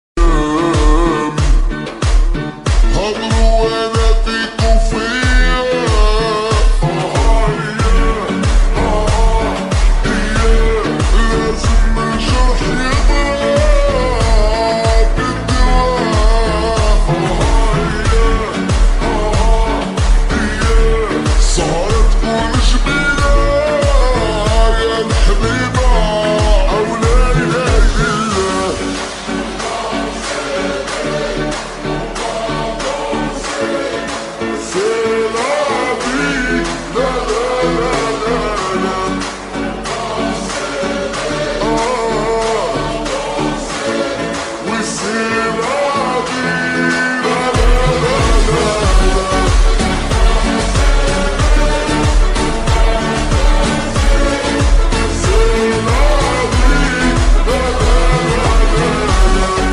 Cristiano Ronaldo vs Messi latest match Riyadh Session Cup 2023 King Fahd stadium
Al Nassr vs PsG Cristiano vs Messi One last Time Riyadh Session cup in King Fahd stadium